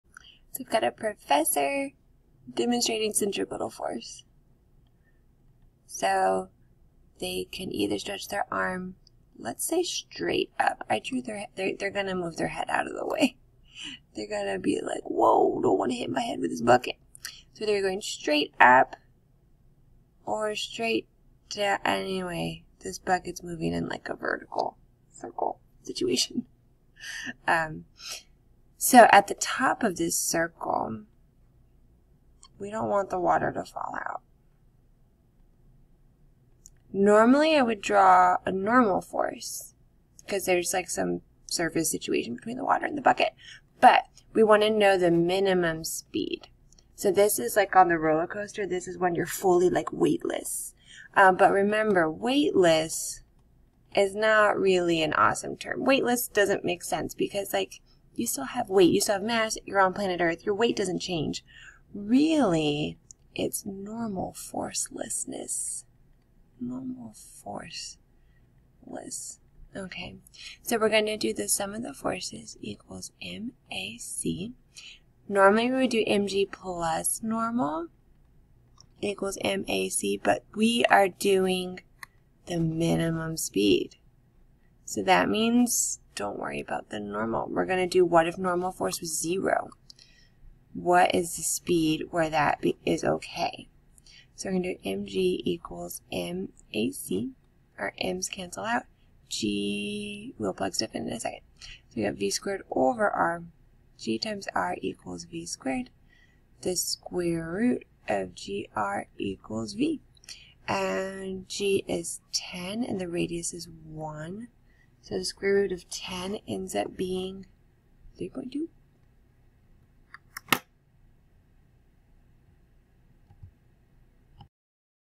Minimum speed bucket of water
A professor likes to demonstrate centripetal force by swinging a bucket of water in the vertical direction. What is the
minimum speed he must swing the bucket at the top of the circle if he is not to get drenched? (Assume that his arm is 1.0
m long.)